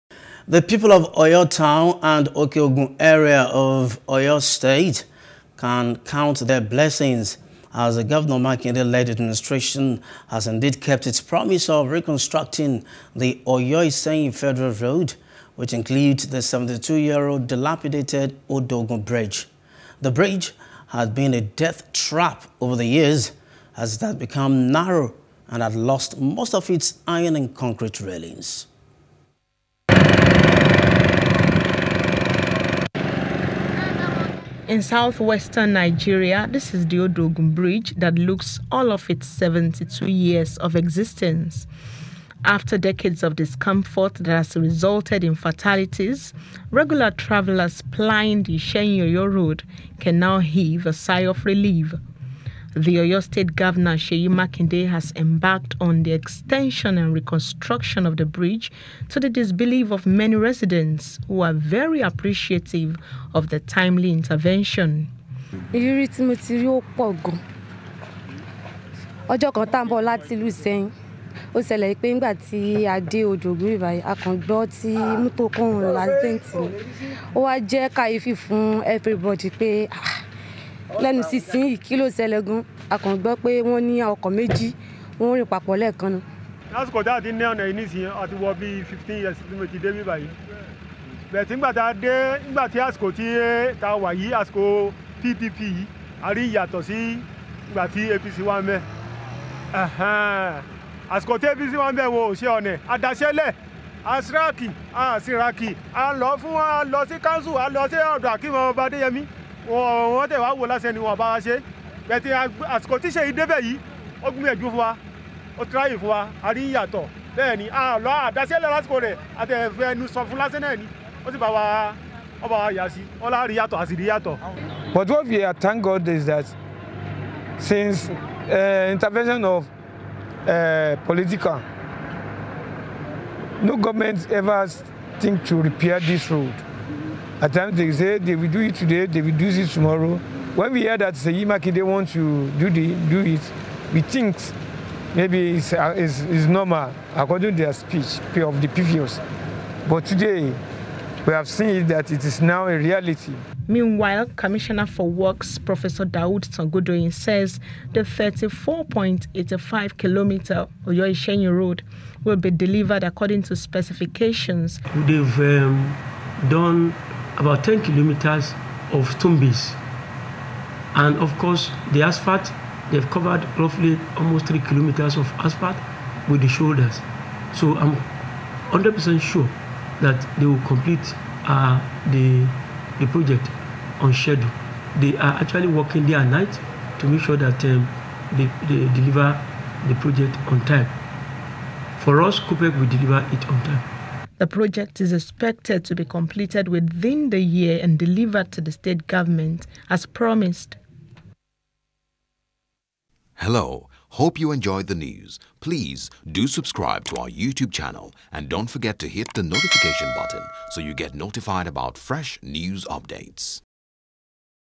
Oyo: Reconstruction Begins On 72 Year Old Bridge, Oyo-Iseyin Road | NEWS
The people of Oyo Town and the Oke-Ogun area of Oyo State can count their blessings as the Governor Makinde led administration has indeed kept its promise of reconstructing the Oyo-Iseyin federal road which includes the 72-year-old dilapidated Odo-Ogun bridge.